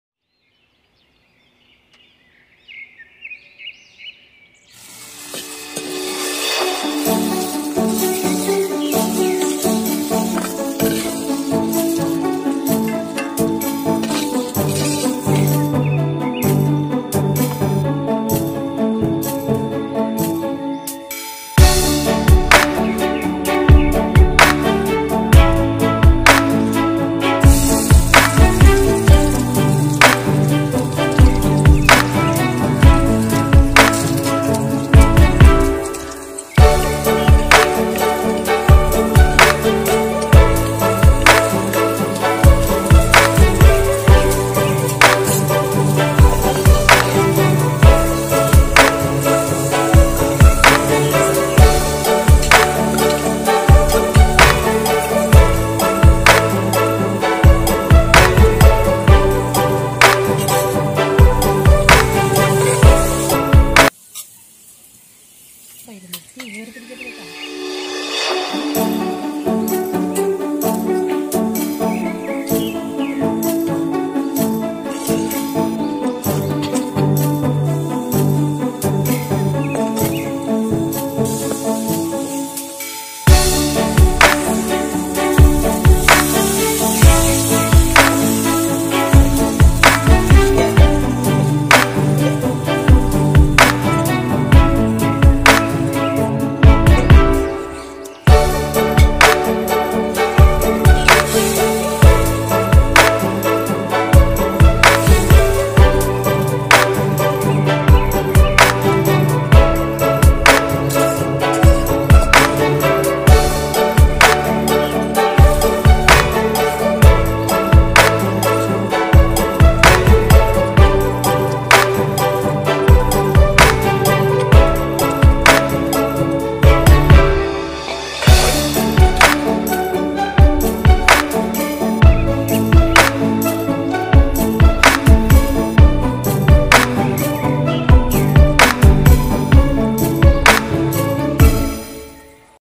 Simple Bhindi masala for chappathi
This is a simple north Indian dish, best combo with Chappathi, prepared by my sis..
Hope you all will try it out and share your experience with me..